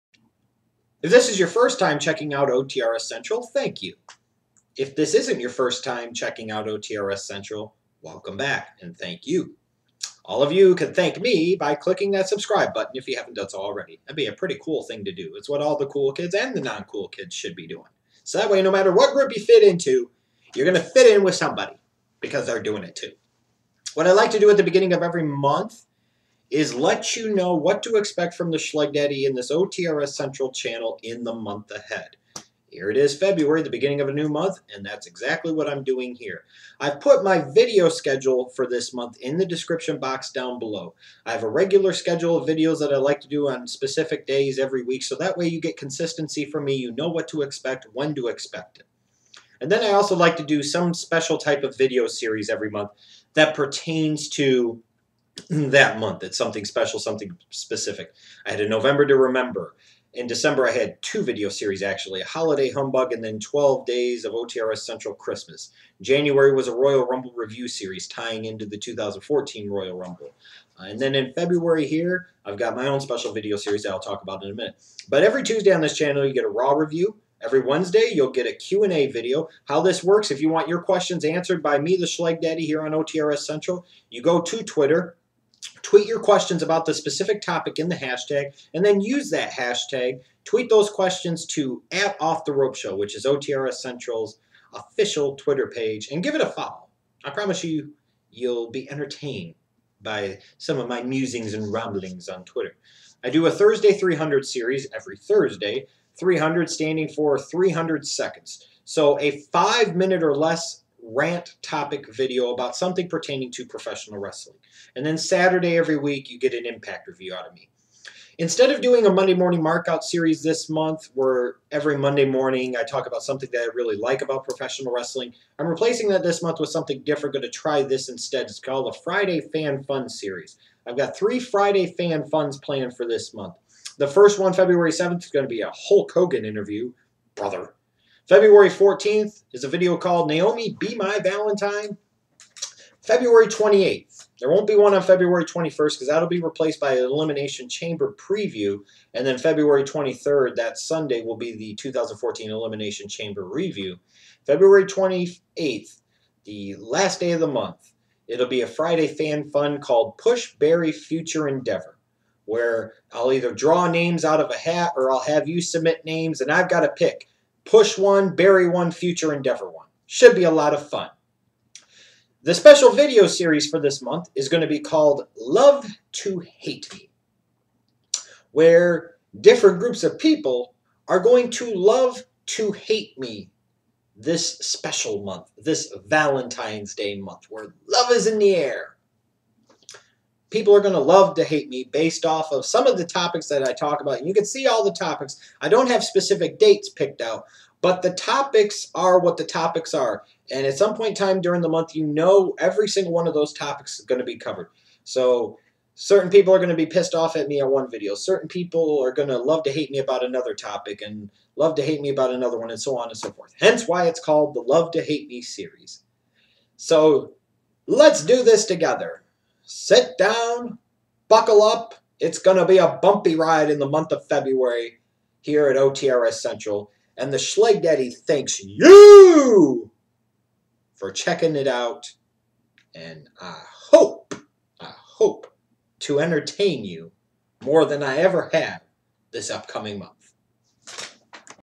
FEBRUARY 2014 OTRSCENTRAL VIDEO SCHEDULE
2/28 Friday Fan Fun: "Push, Bury, Future Endeavor"

LOVE TO HATE ME VIDEO SERIES TOPICS:
(Randomly Done Throughout The Month of February)

To Hell With Chris Benoit
Kayfabe:  All the Way or Go Away